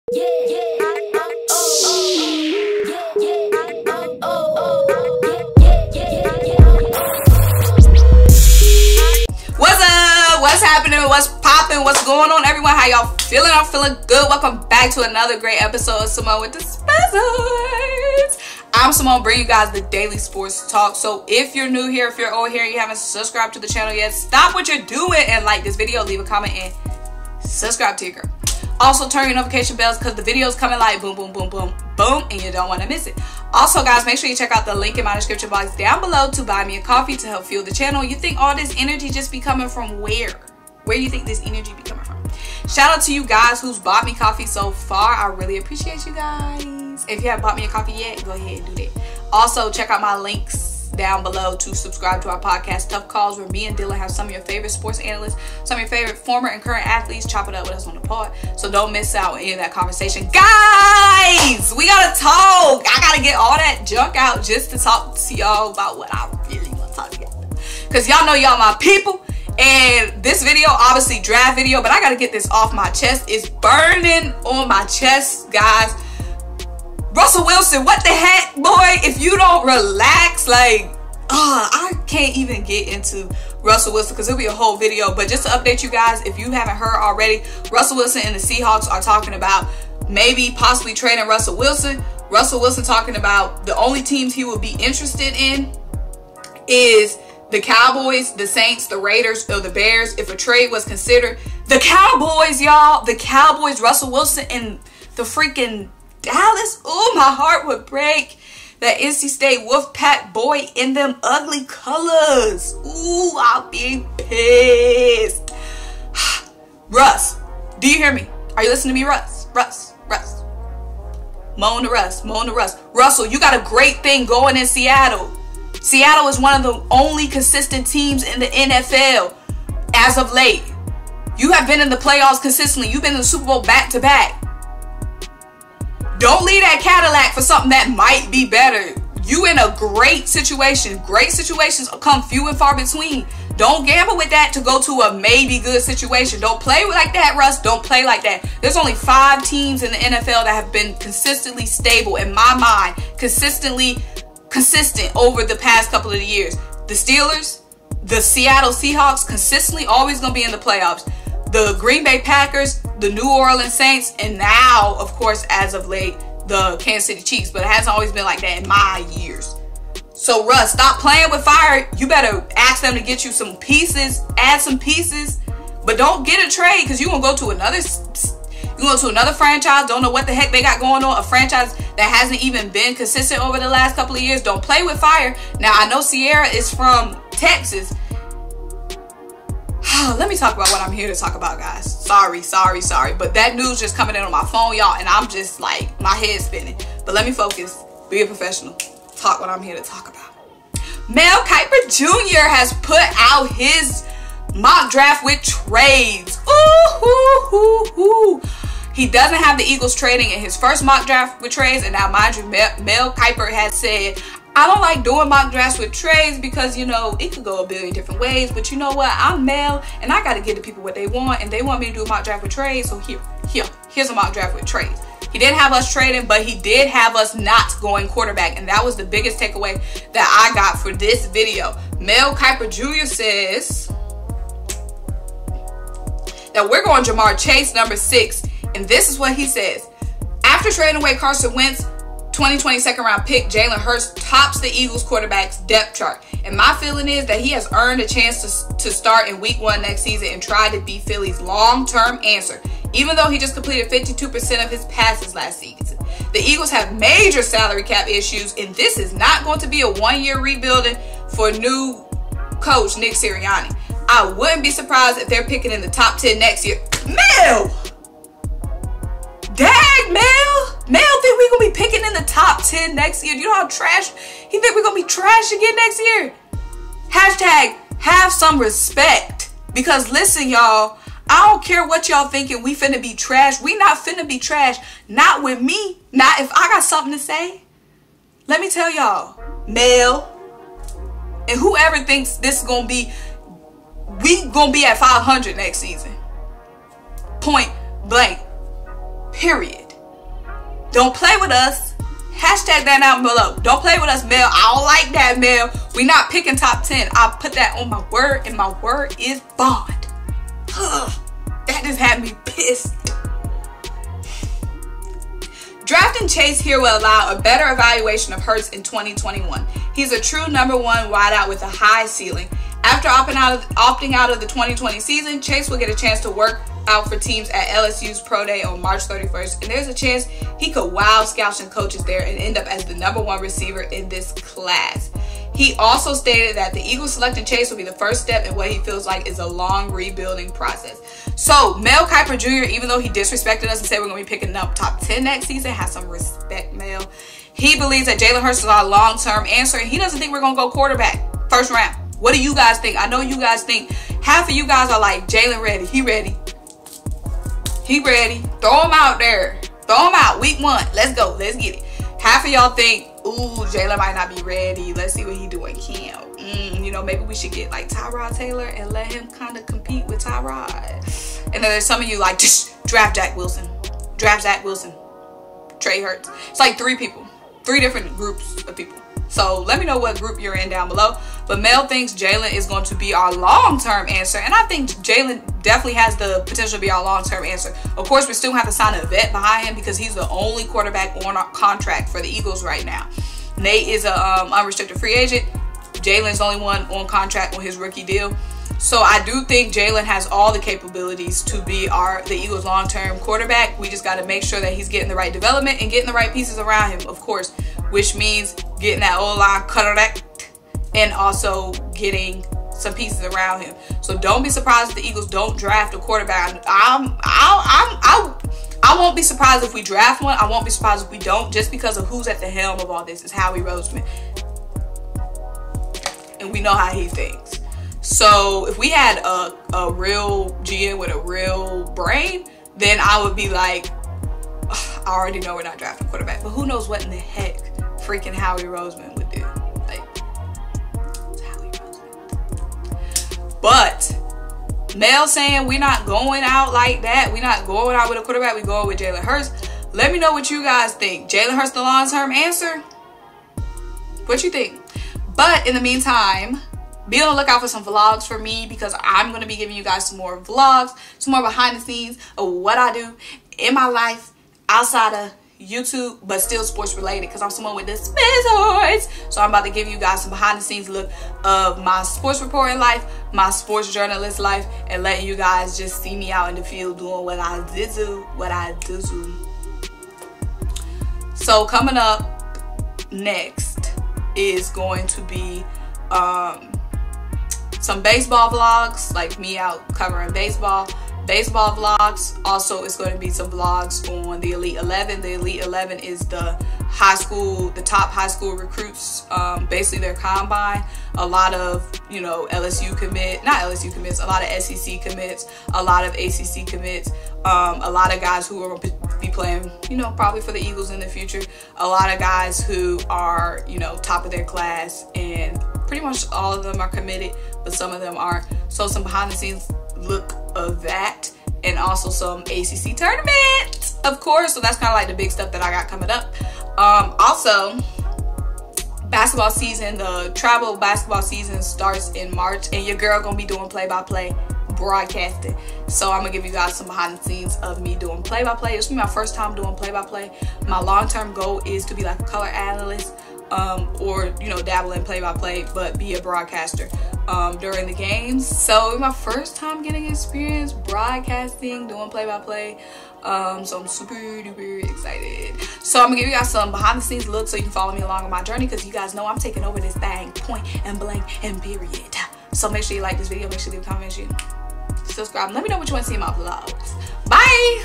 PHILADELPHIA EAGLES MEL KIPER MOCK DRAFT WITH TRADES | JALEN HURTS THE FUTURE?
WHATSSSSSSSSSSS UPPPPPPPPPP

HMU!!

CARSON WENTZ TRADED !?! |